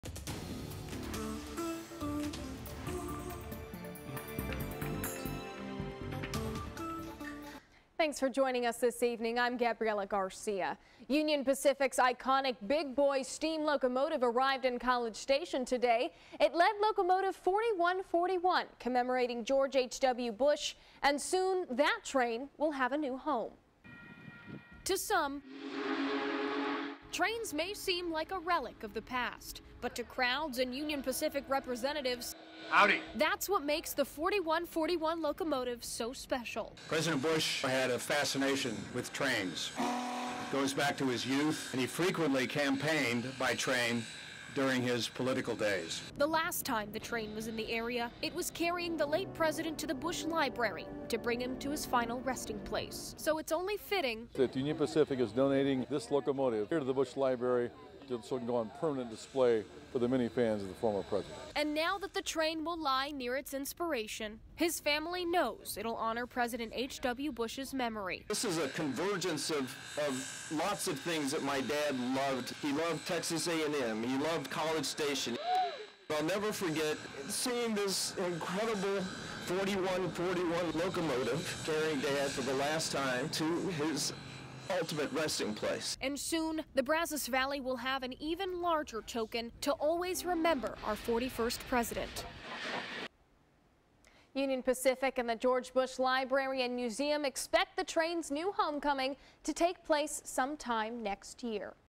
WHOO WHOO! 4141 Bush locomotive to make College Station its permanent stop
The announcement was made Friday in front of a local crowd who got to see the iconic locomotive up close.